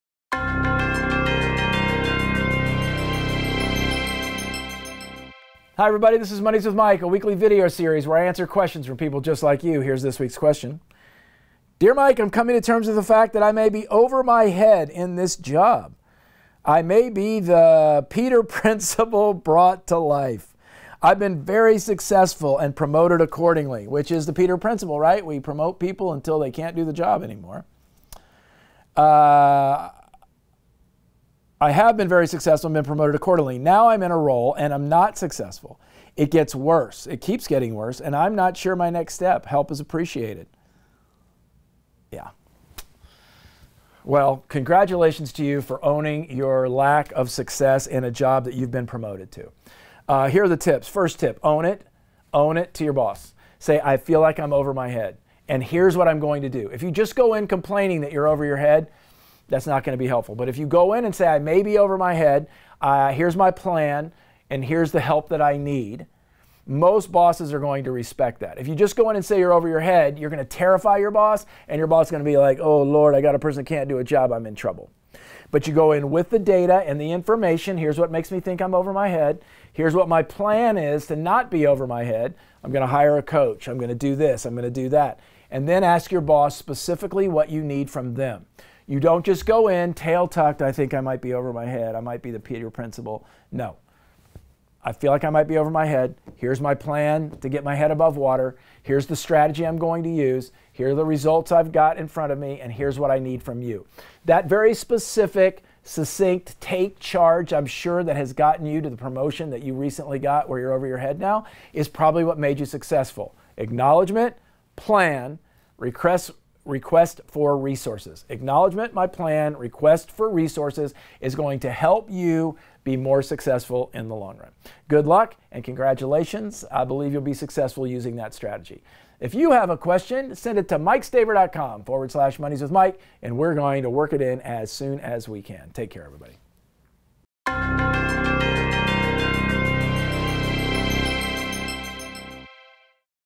Mondays with Mike: Promoted Beyond Your Skills
Mike Staver responds to a viewer who is feeling overwhelmed and underprepared to do their current job.

Have a question that you’d like Mike to answer?

Until next time, be courageous!